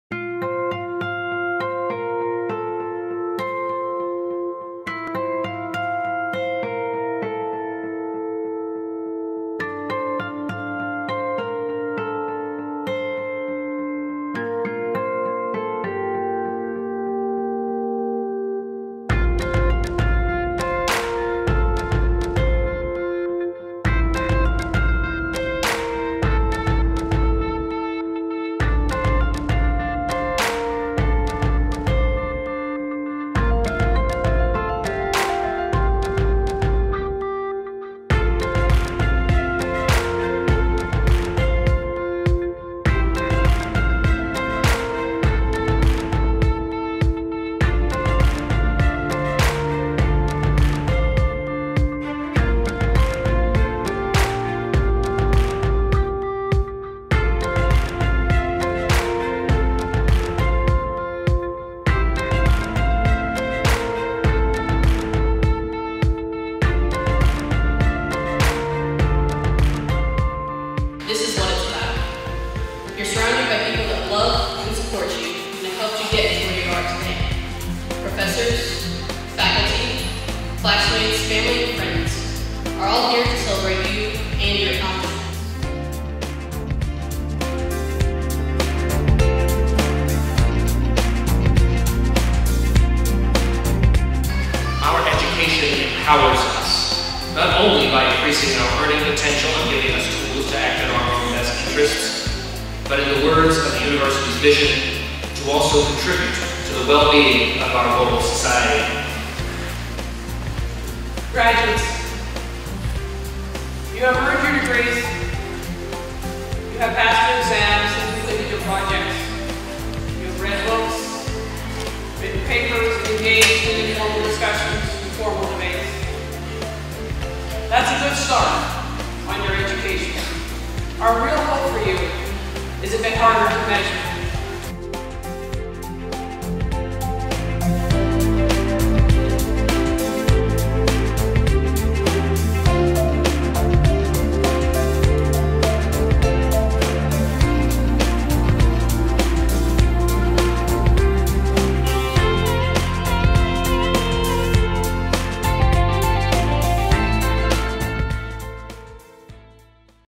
University of Saint Mary 2014 Graduation Ceremony
Congratulations to the University of Saint Mary graduating class of 2014.

Go out there and use your God-Given potential to make a lasting impact on our world!